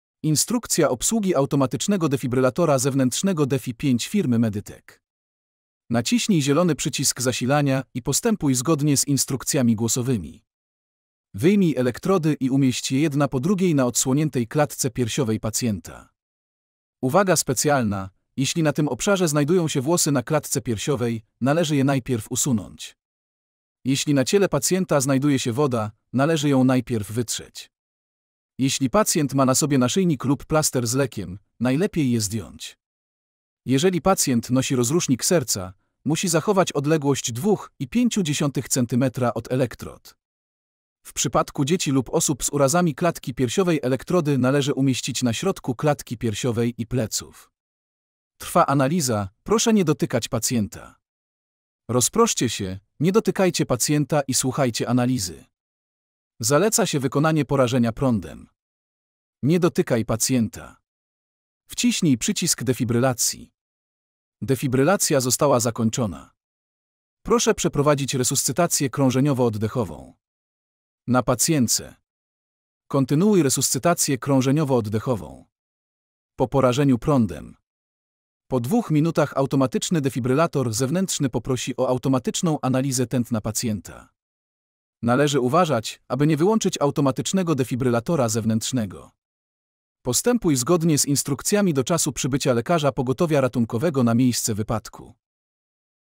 Dlaczego każda firma powinna zainwestować w automatyczny defibrylator zewnętrzny (AED)?
W dzisiejszym dynamicznym i nieprzewidywalnym świecie bezpieczeństwo i dobre samopoczucie pracowników, klientów i gości są najważniejsze.💯🔝

Chociaż wiele firm priorytetowo traktuje bezpieczeństwo przeciwpożarowe, apteczki i wyjścia awaryjne, jednym z kluczowych aspektów, który często jest pomijany, jest dostępność automatycznego defibrylatora zewnętrznego (AED).⚡️

Po więcej informacji:-